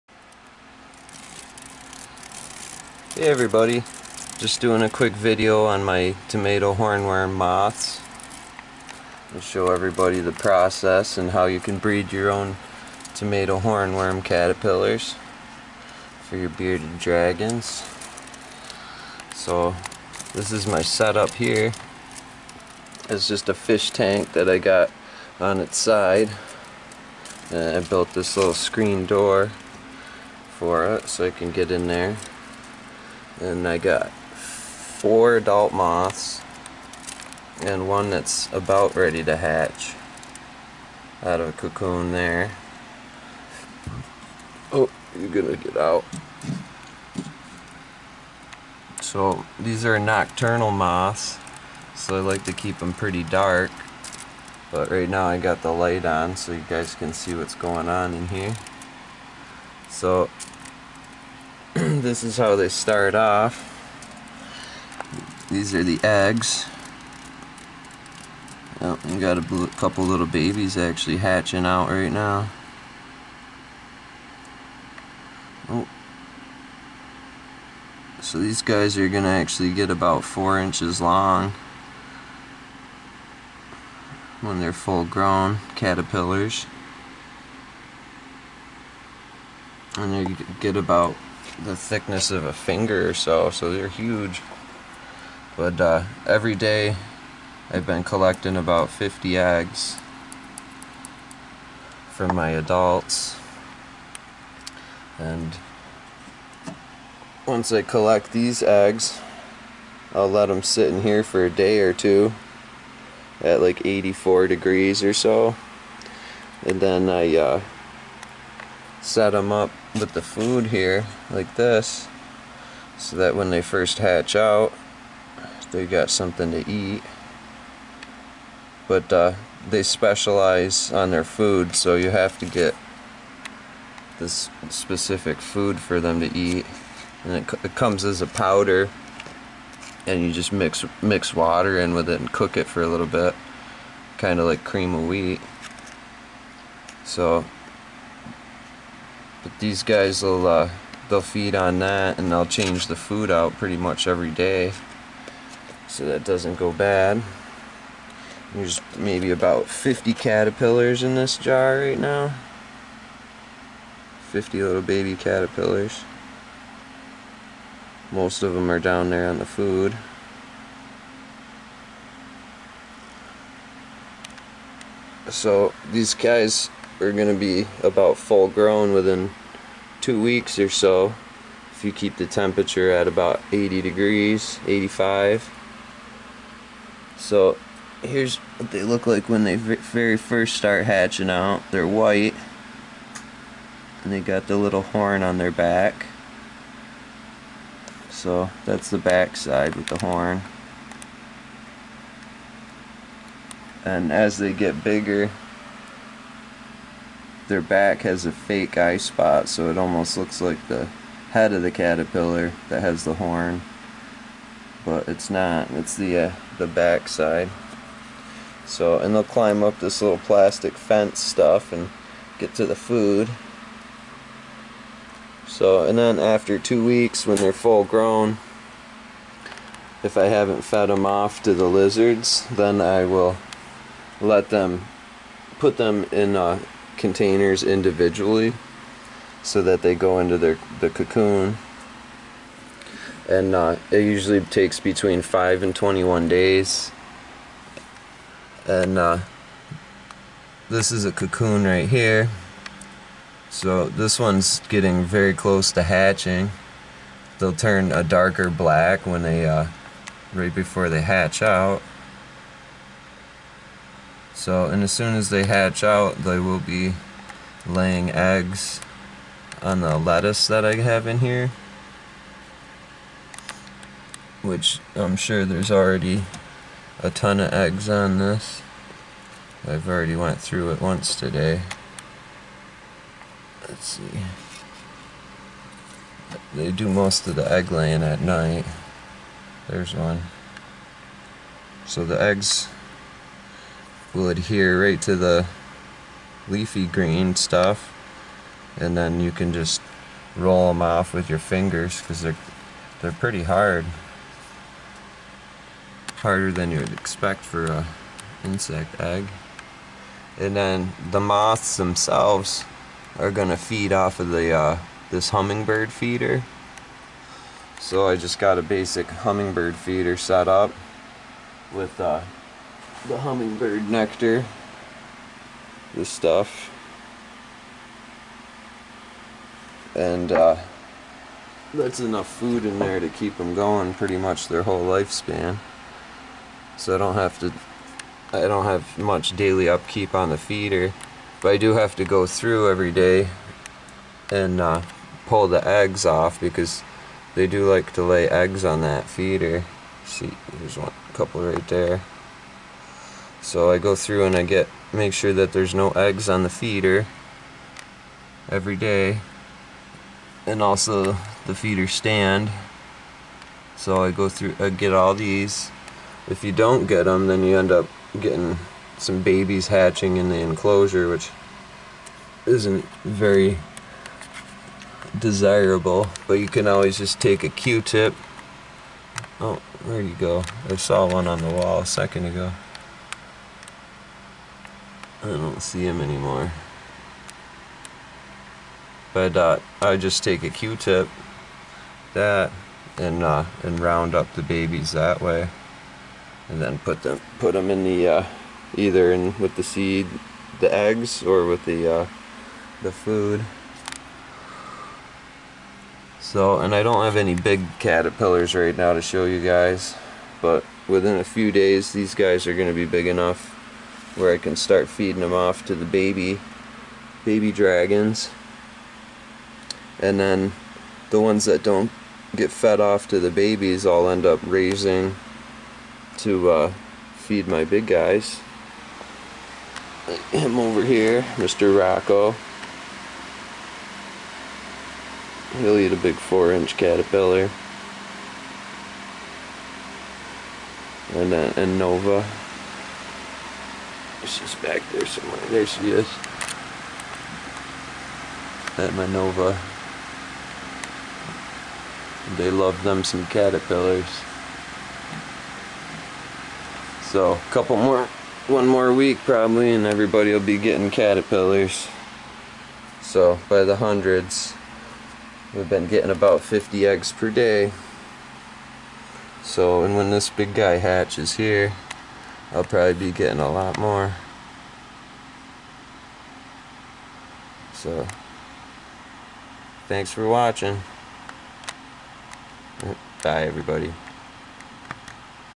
Raising Tomato Hornworm Caterpillars for my Dragons!
A short demonstration video on how I raise my own tomato hornworm moths and caterpillars to feed my reptiles.